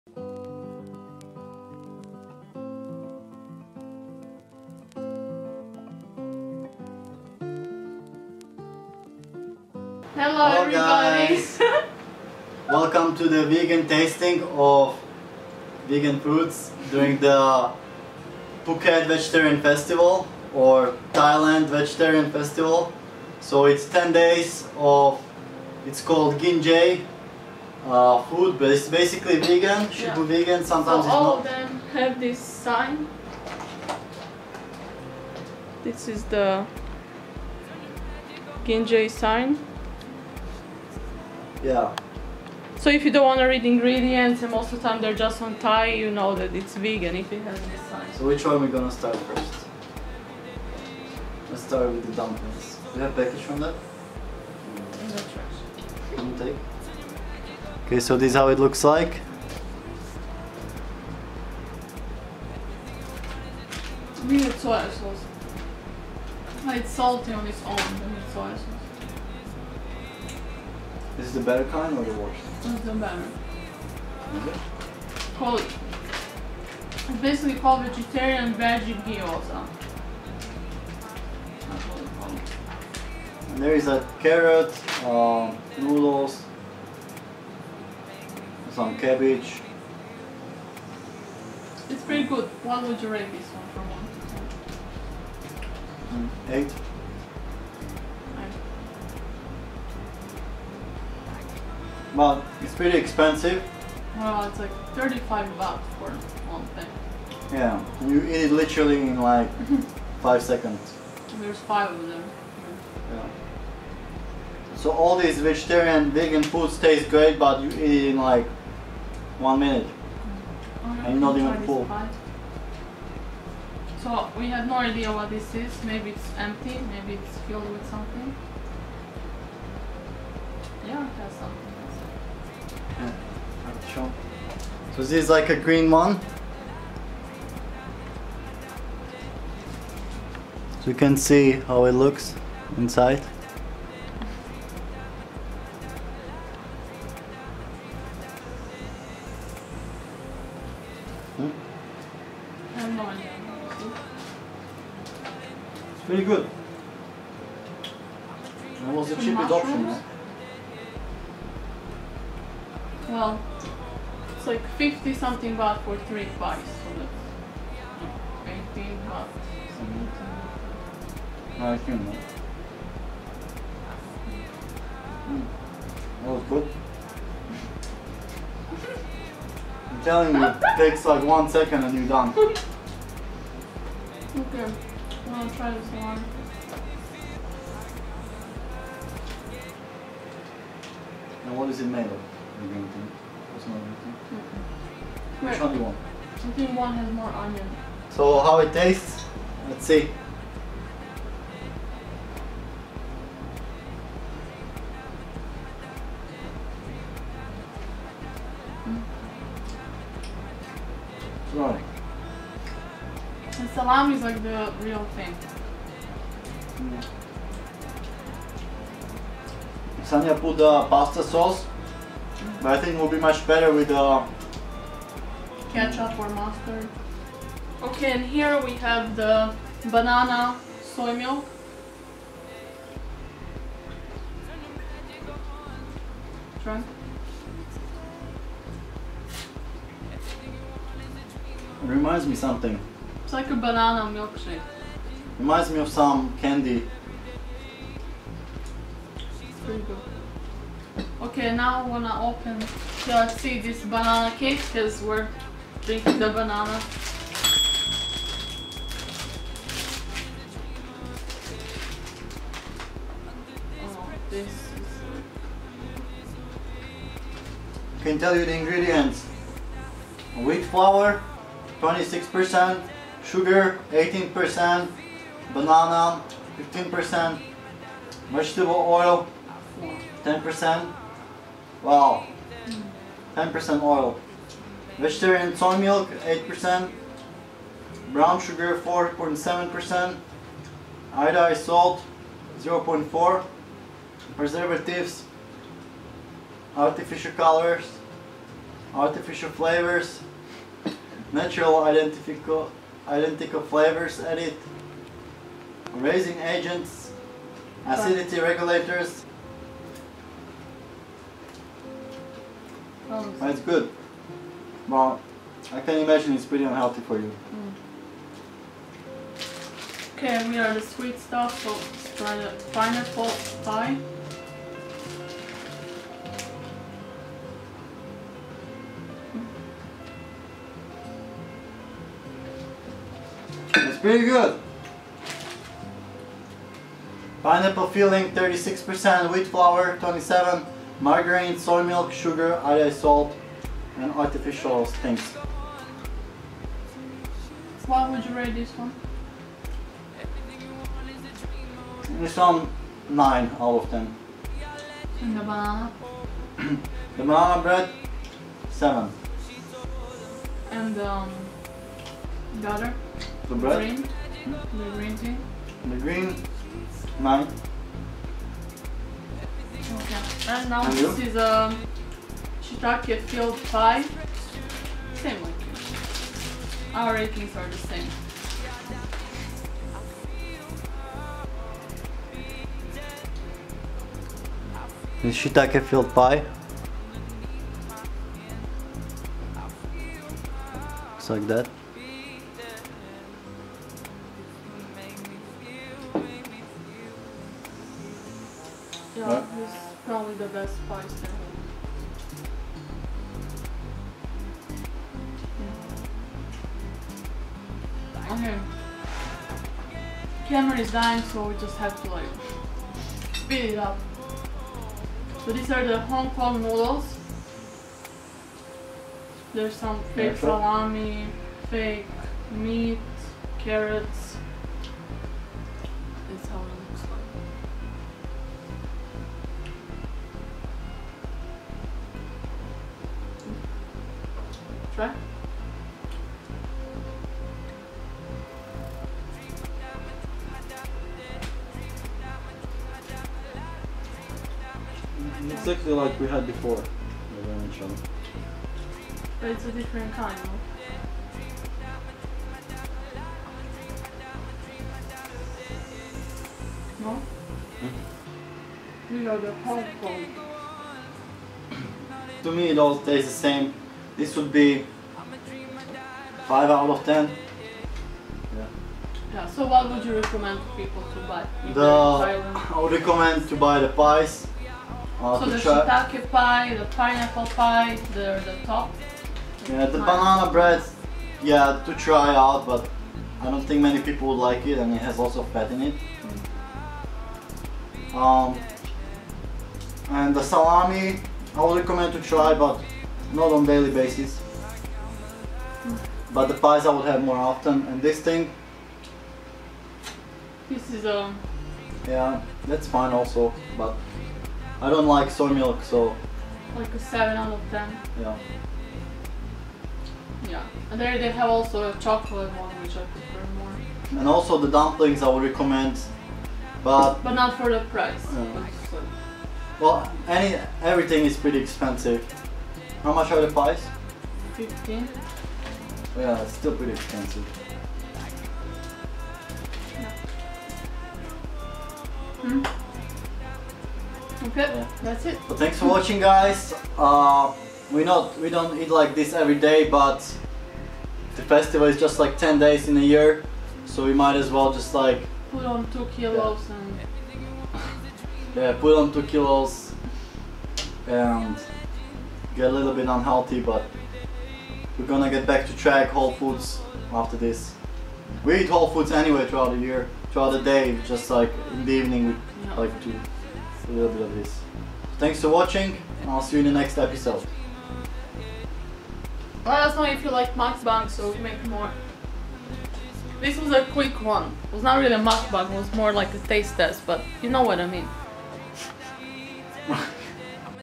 7Eleven Vegan Foods Taste Test
Vegan options in 7Eleven during the Vegetarian Festival in Thailand.